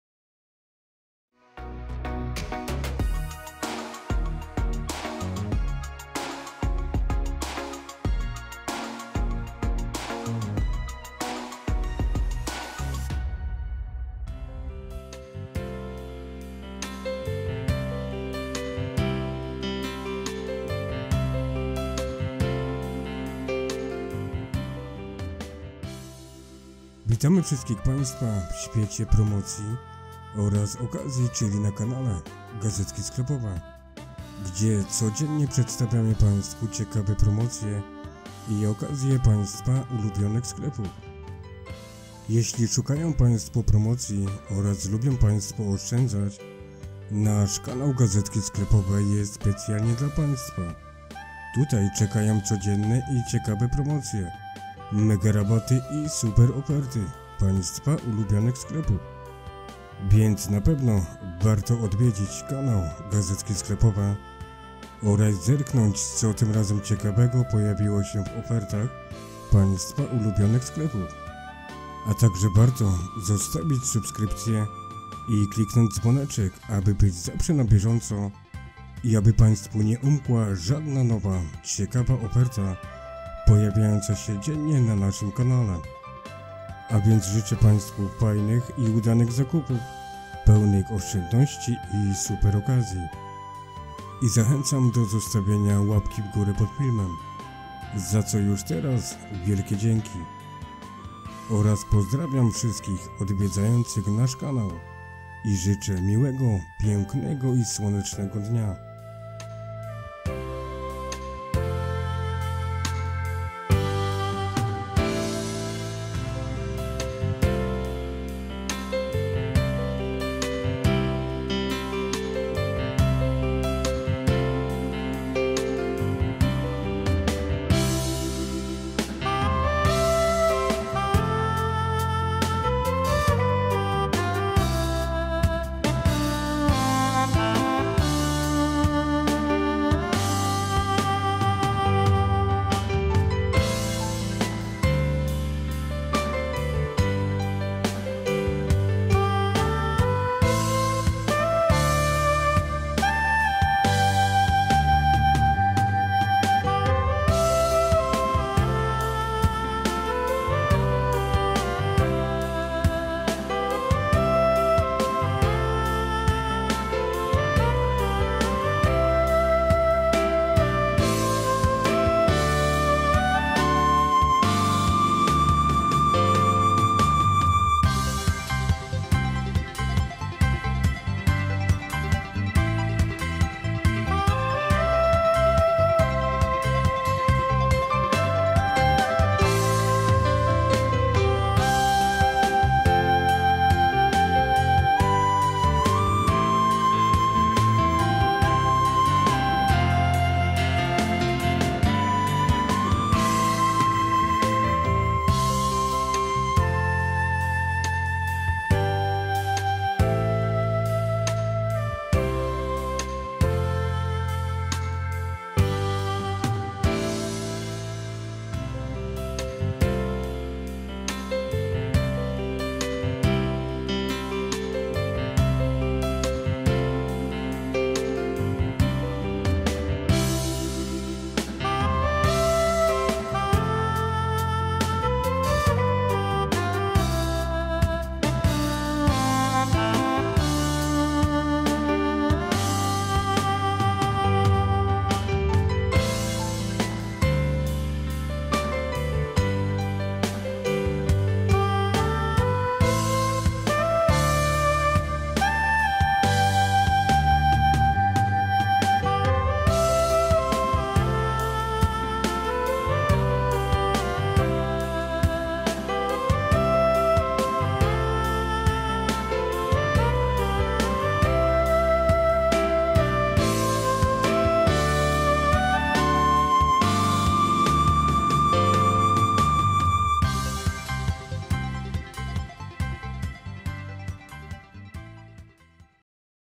Action | Nowa Oferta-Gazetka Już Od Środy 10.08.2022 | Dla Ciebie – Dla domu - W Dobrych Cenach
Aktualne Promocje Action Oferta Od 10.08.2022 Do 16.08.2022 – lub do wyczerpania zapasów
Action to oferta przydatnych artykułów przemysłowych w promocyjnych cenach. Zajrzyj do swojego sklepu Action i wybierz coś dla siebie.
W ofercie tej na pewno znajdziemy wiele fajnych ofert przystępnych cen mnóstwo promocji oraz wiele ciekawych propozycji myślę że warto również zaglądnąć do tej oferty Co dokładnie znalazło się tym razem? z pewnością dowiemy się oglądając niniejszy film